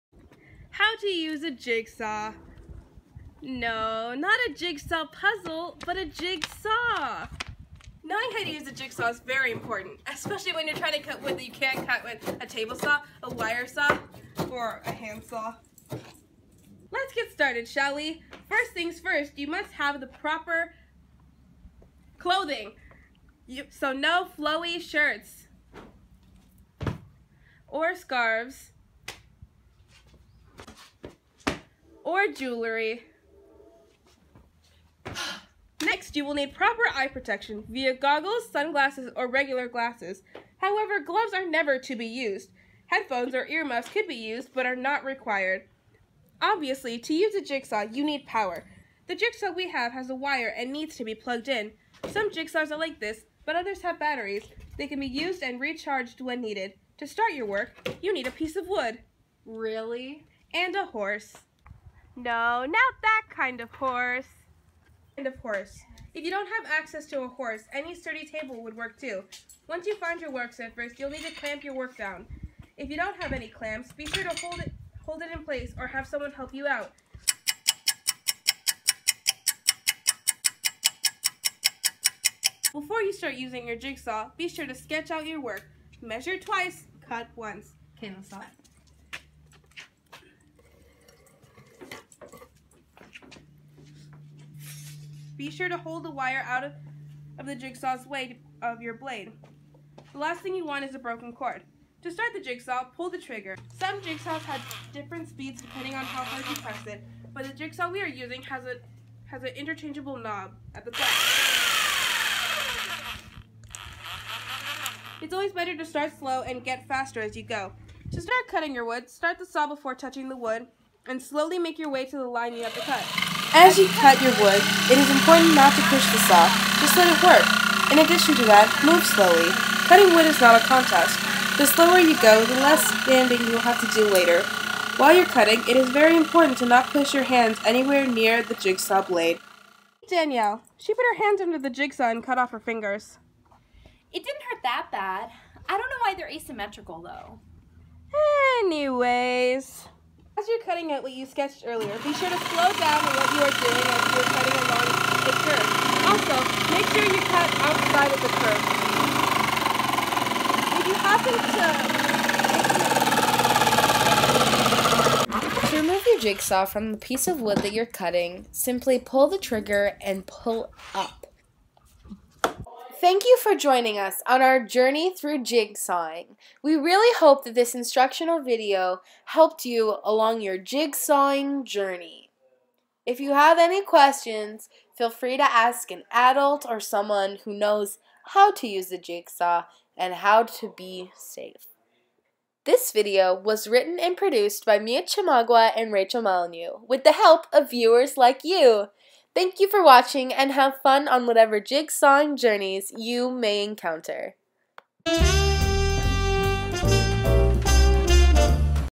Jigsaw Safety 101
In this video, we demonstrate how to correctly and safely operate a Jigsaw.